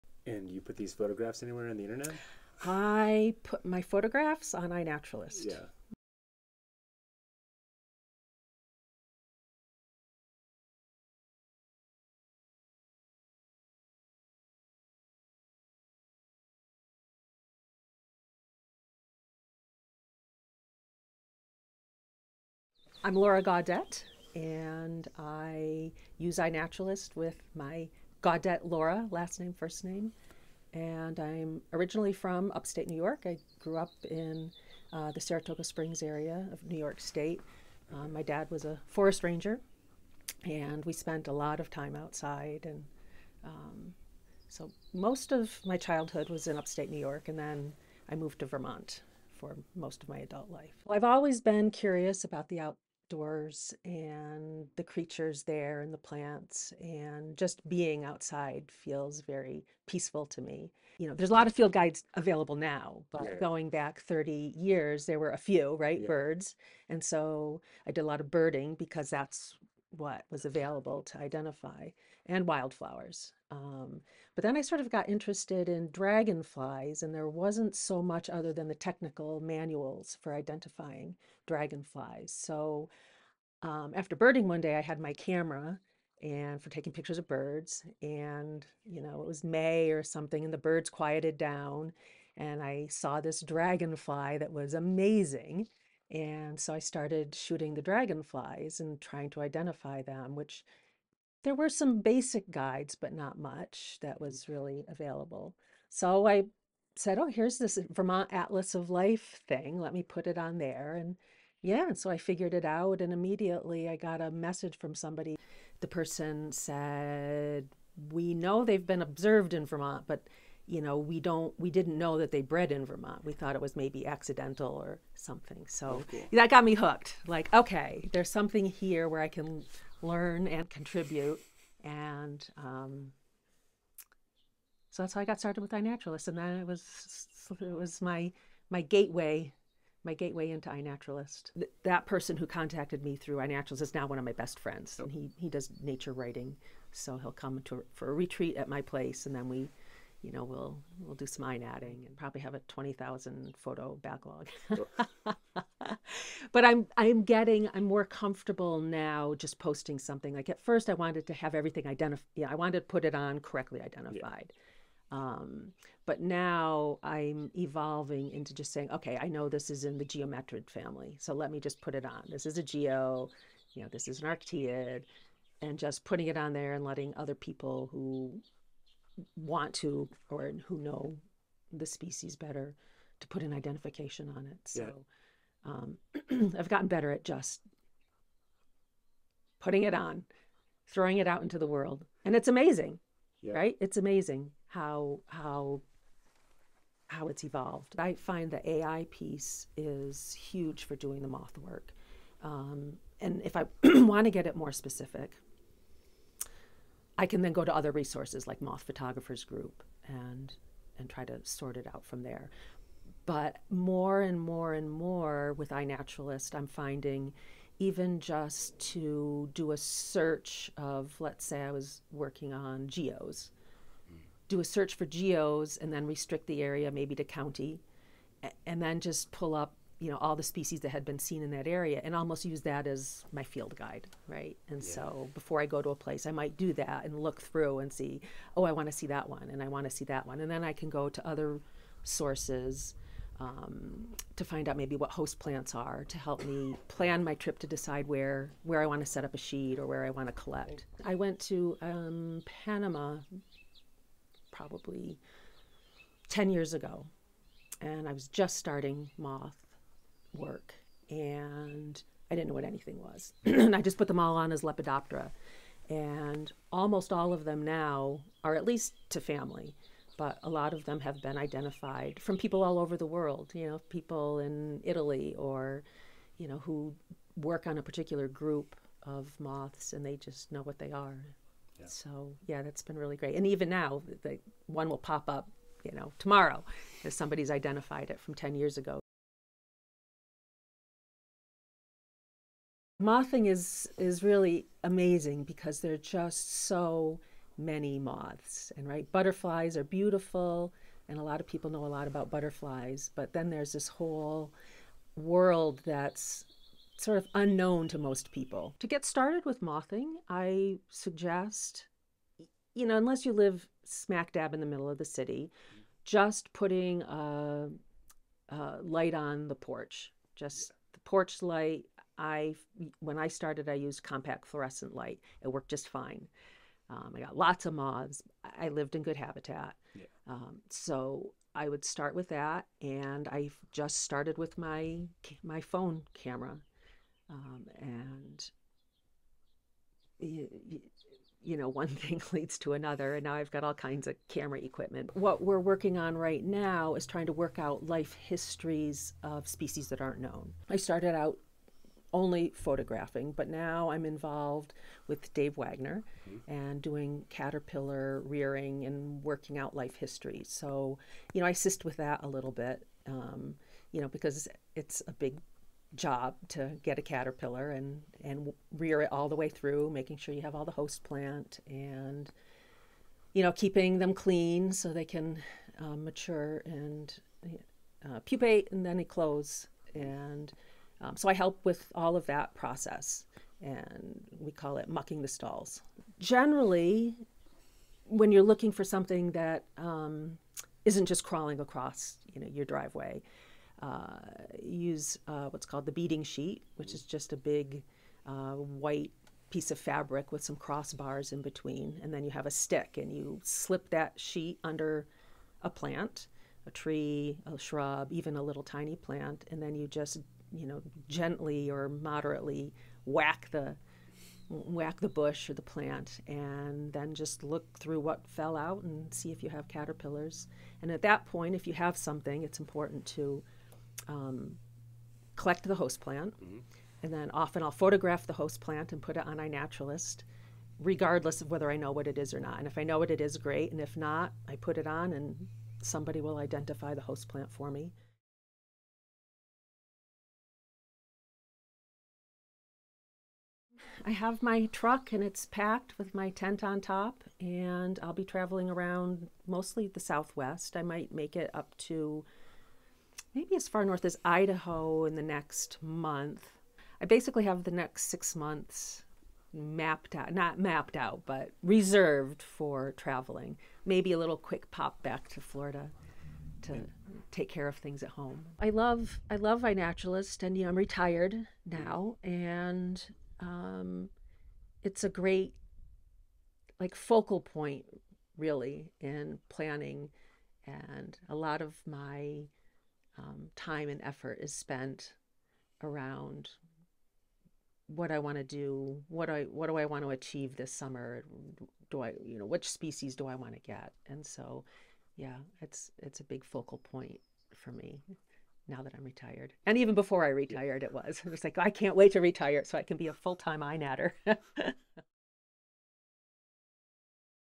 Mucking the Stalls: An Interview About Caterpillar Research, Mothing, and iNaturalist
This spring, we caught up with iNat user Laura Gaudette (gaudettelaura on iNat), who was helping Professor Dave Wagner and others research caterpillars in southwestern United States. We discussed her history with iNaturalist, how to start mothing, and the finer points of using a beat sheet.